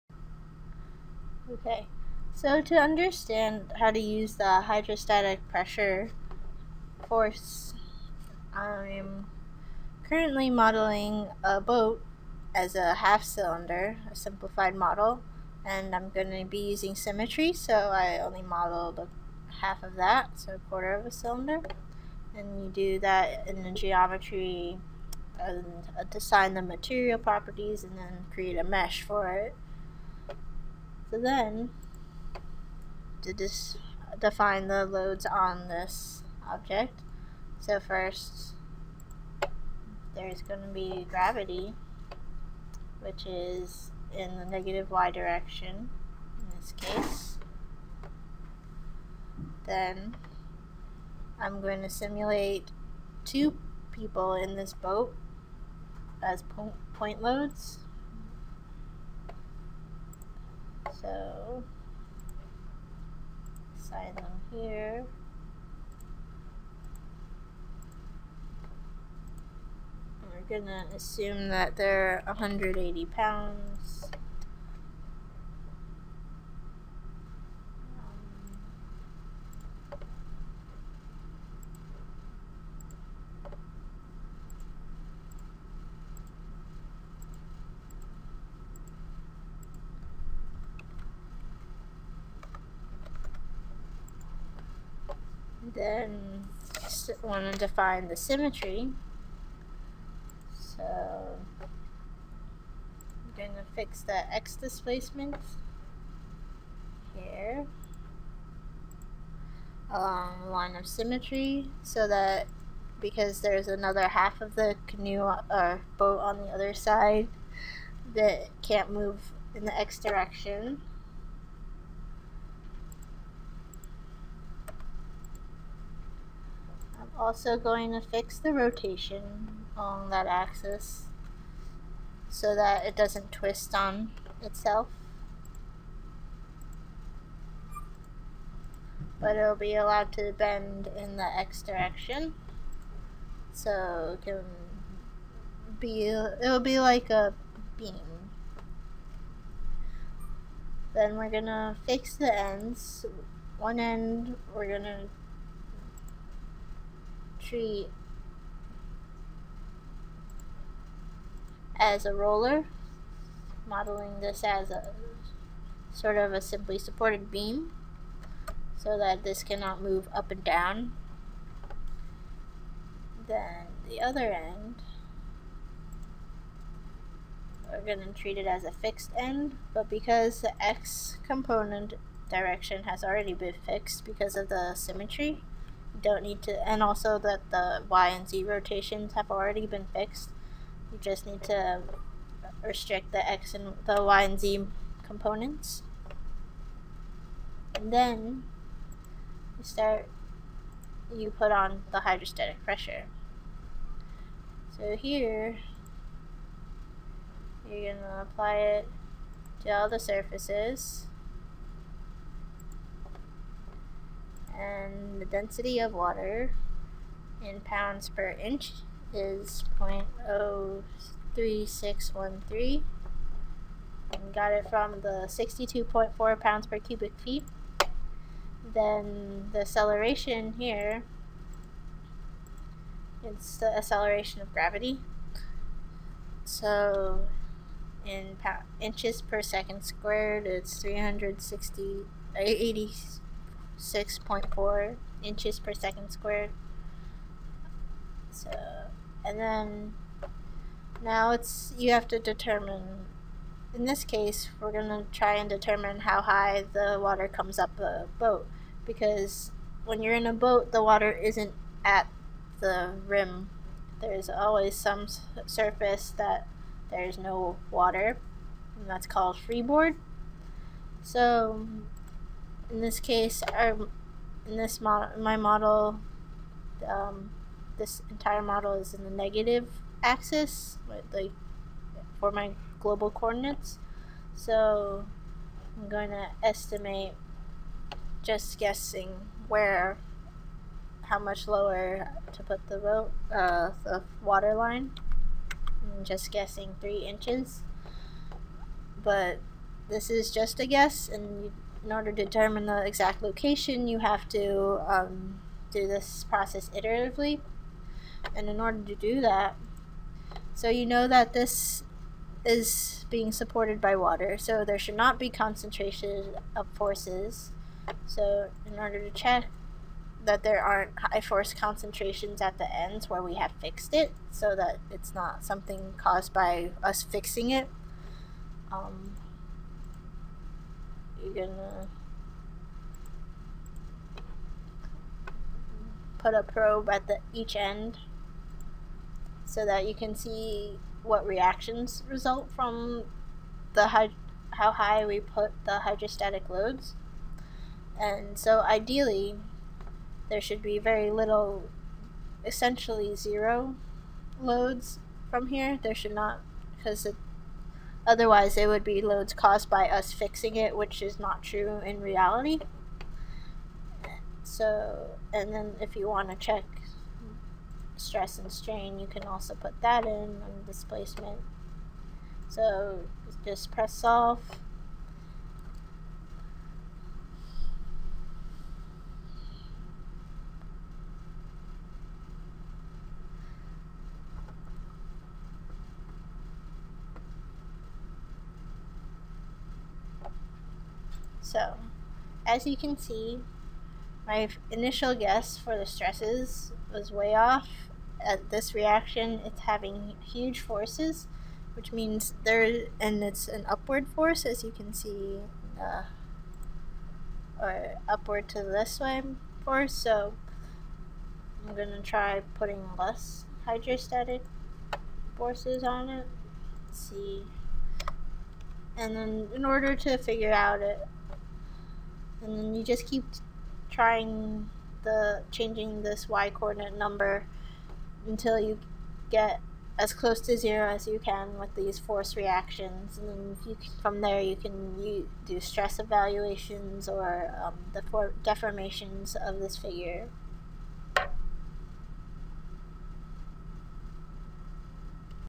Hydrostatic Pressure tutorial
Tutorial on how to use the hydrostatic pressure in ANSYS Workbench to determine the height of the water line.